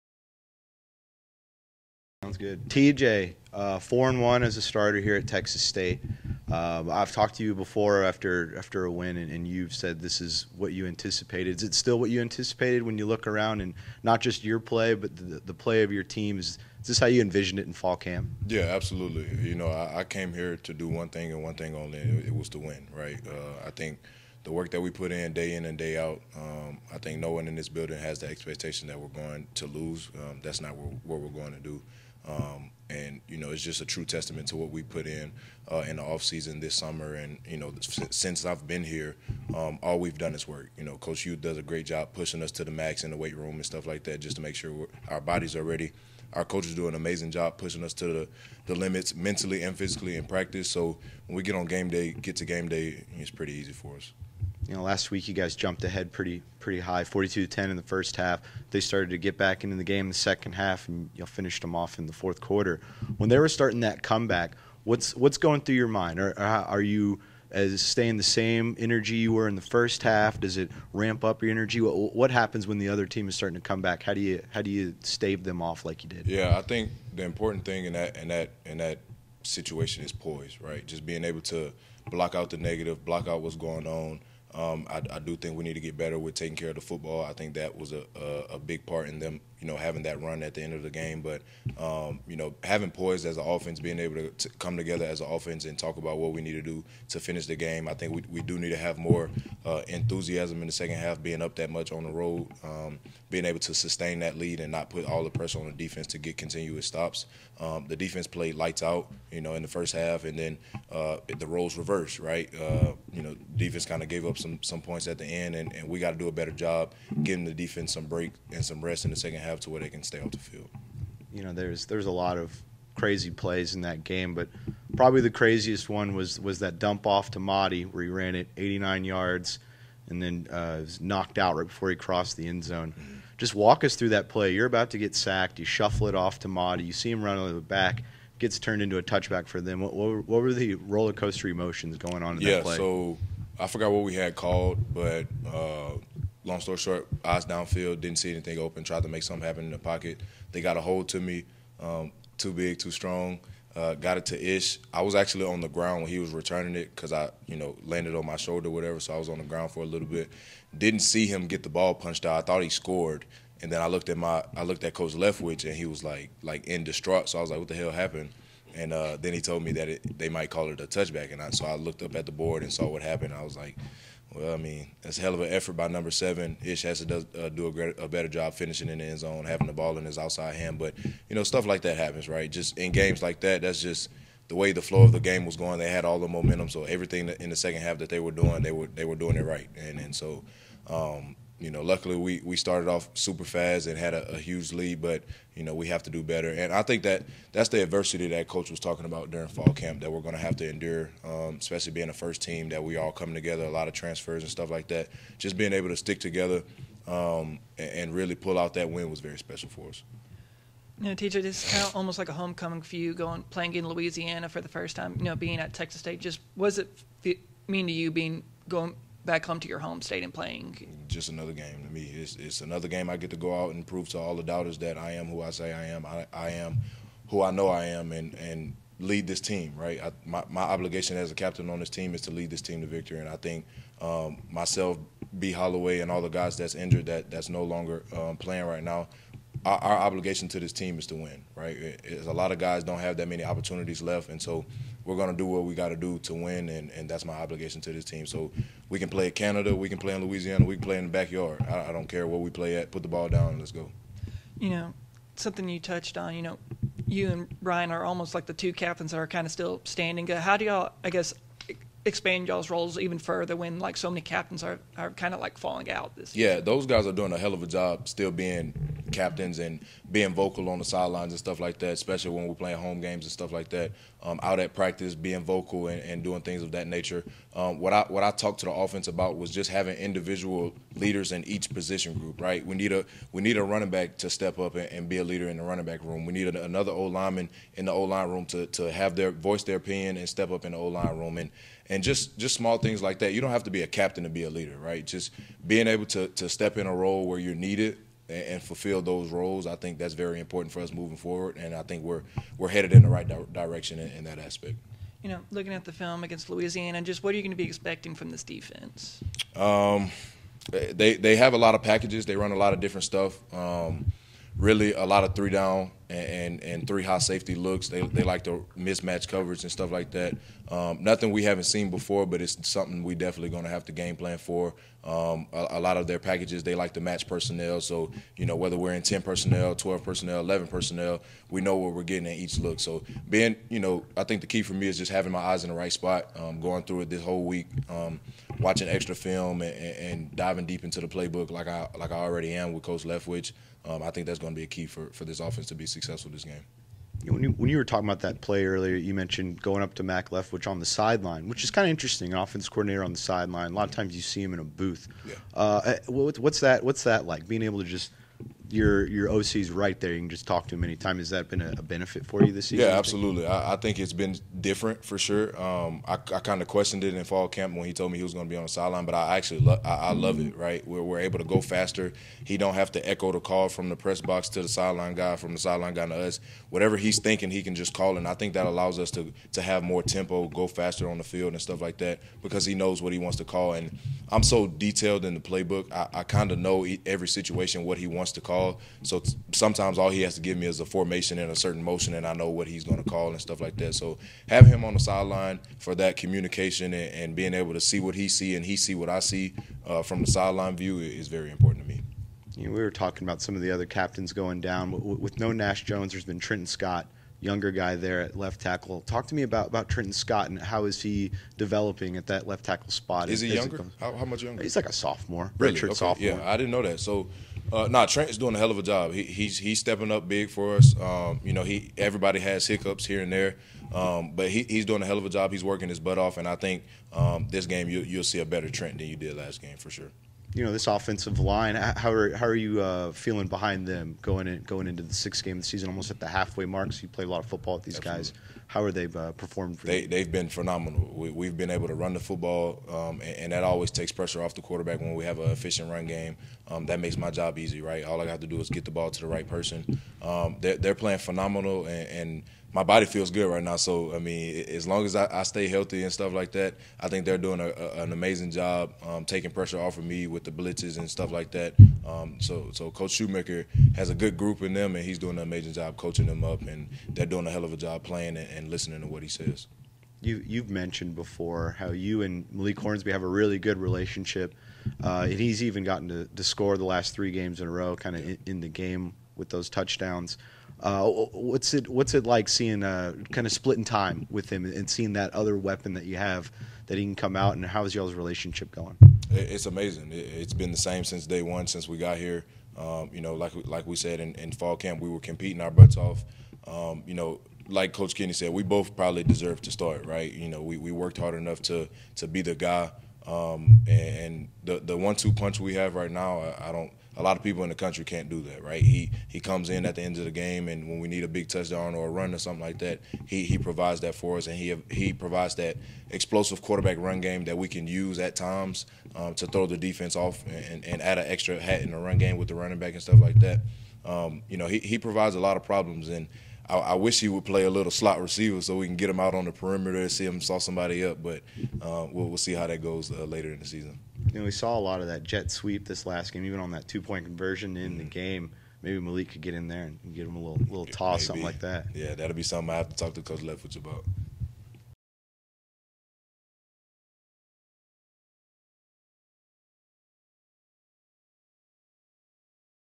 FB | QB TJ Finley Week 6 Press Conference (Oct. 3, 2023)
Texas State quarterback TJ Finley talked with the media after practice on Tuesday ahead of the road game at Louisiana on Oct. 7 (2:30 pm, ESPNU).